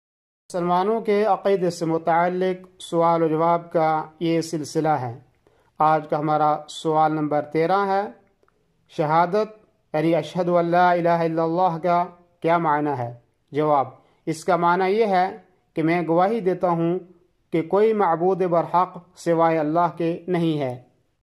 Daily Q&NA||Q:13||Aqeedah Tawheed||By Scholar M Yaseen Madni
سوال 13:__ شھادۃ ان لا الہ الا اللہ کا کیا معنی ہے؟
جواب :_ اس کا معنی یہ ہے کہ نہیں کوئی معبود برحق سوائے اللہ کے ۔